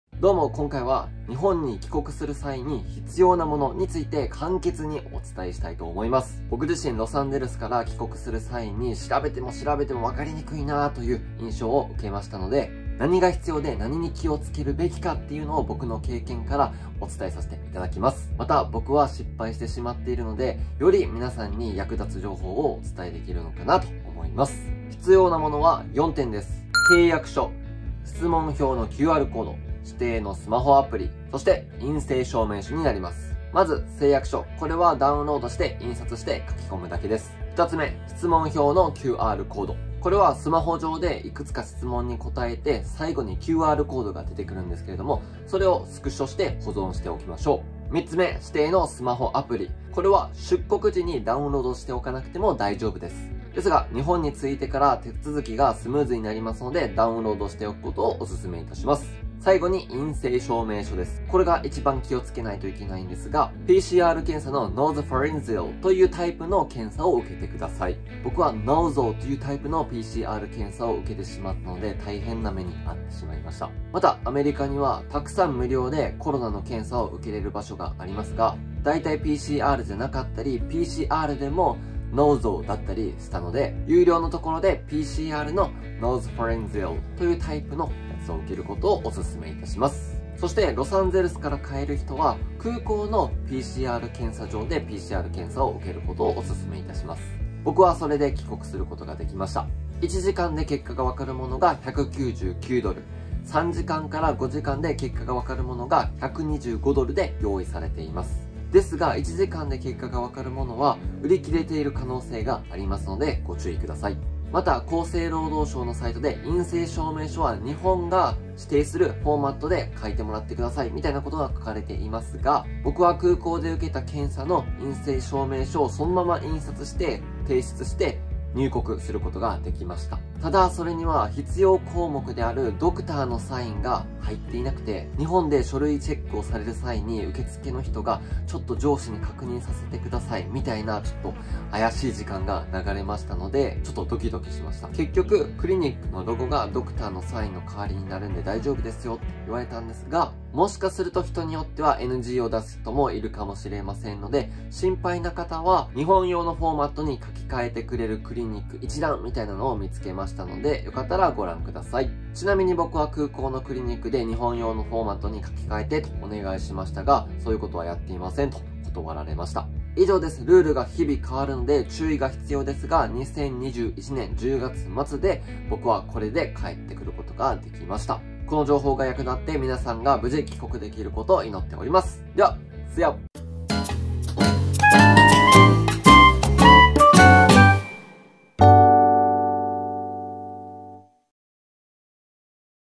【超わかりやすい】コロナ禍に海外から日本帰国する方法(特にアメリカ・ロサンゼルスから帰る人にオススメ)
追伸:アメリカってどこで印刷できるの！？と僕も思いました。空港で、Can I ask you a favor? Could you print this out for me?とPDFを見せて言えば大丈夫です！

★ロサンゼルス空港PCR検査を受けたクリニックのサイト
★日本が指定する陰性証明書フォーマット(念のため貼っておきます)
★ロサンゼルスで日本フォーマットに書き換え対応してくれるクリニック一覧

～チャンネル内の人気動画～
☆【出川イングリッシュ】オーストラリアのスタバでカタカナ英語は通じるのか？